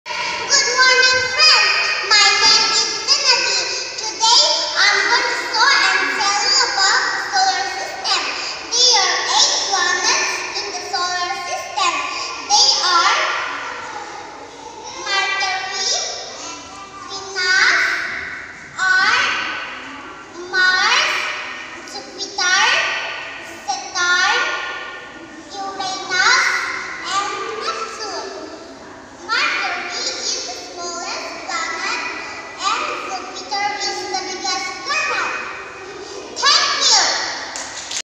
presentation on the eight planets
This  video is about presentation done by 7 years old girl on the topic  "The eight Planets of our solar system" in the Spring meet  programme organised by the school, North Eastern Children's Academy, Rengkai, churachandpur.